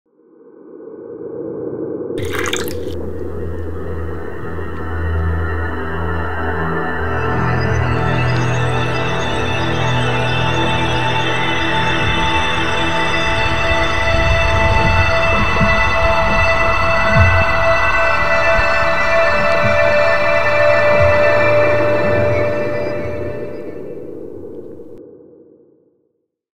The Magic Aurora Cup Luleå
The Magic Cup Aurora is a unique mug that features a beautiful aurora borealis (northern lights) design. When the mug is cold, it appears black, but when you fill it with a hot beverage, the stunning aurora image gradually appears.

This particular design showcases the northern lights over Luleå city in Swedish Lapland.

It’s a great way to add a touch of magic to your morning coffee or tea!